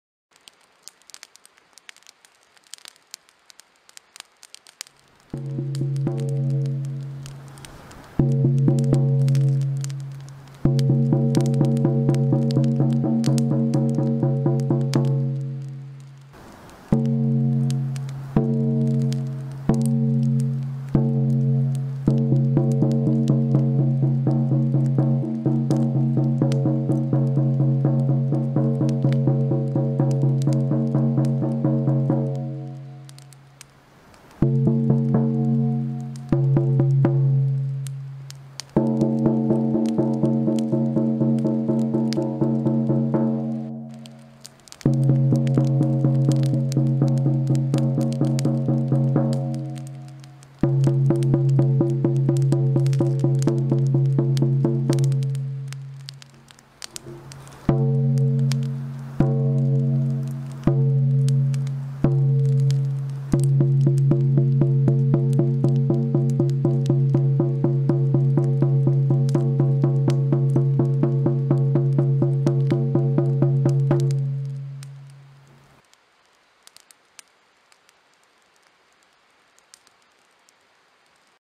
Inuit Şaman Davulu Nedir -Türkiye'de İlk
Uzun zamandır kendim için Inuit davullarını araştırıyordum. Ses tonu ve frekanslarının çeşitliliği ilgimi çekiyordu. En nihayetinde bir tane hazırladım. Kulağın işitebildiği en az üç ayrı frekans ve tınıyı net olarak bu davulda fark ettim. Tutma sopasının yapısı nedeniyle orta kısımdan da yüksek titreşim alabildim. Bu davulun titreşim ve tonlarını en net şekilde kulaklıkla dinleyerek alabilirsiniz.
Kızılderili davulunda olduğu gibi; bu davulu da Türkiye'de ilk kez ben yaptım ve Arctic kültüre ait bir davul ruhunu ve tınılarını burada ilk kez duyurmuş olmaktan mutluyum...
Qilaut sacred wind drum
photographer Andrey Shapran